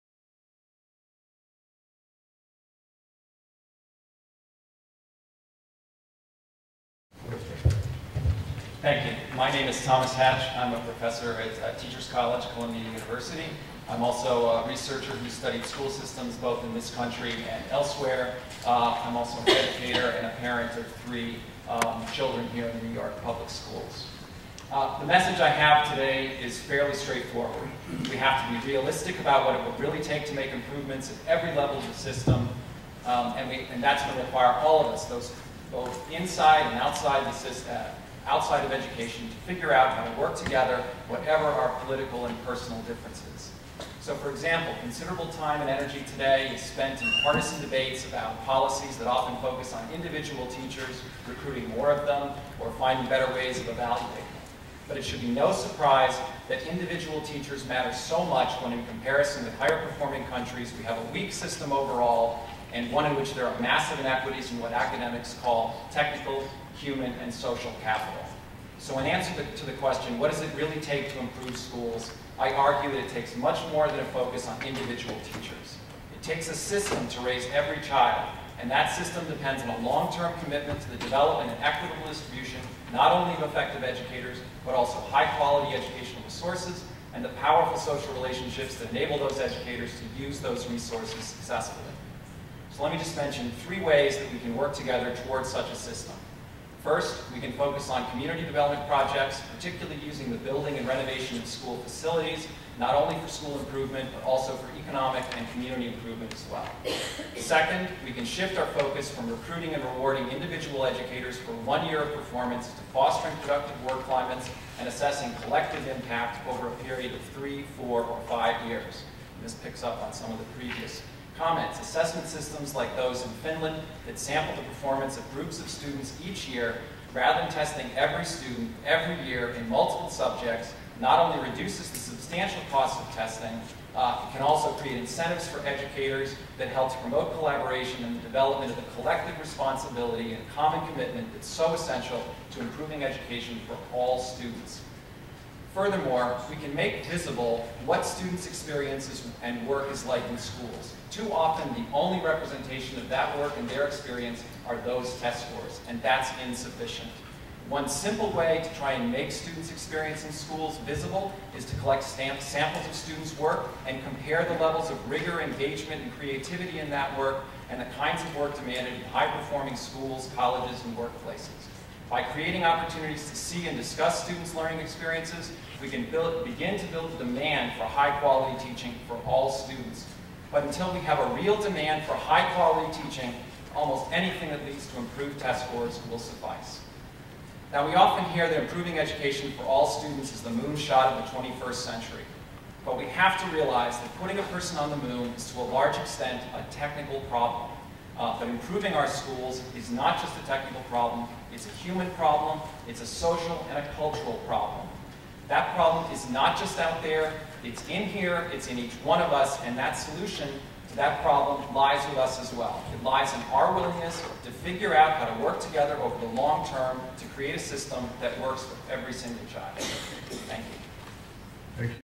NY Ed 02 Hatch
Thomas Hatch at NY Education Reform Commission, Newburgh NY, September 10, 2012.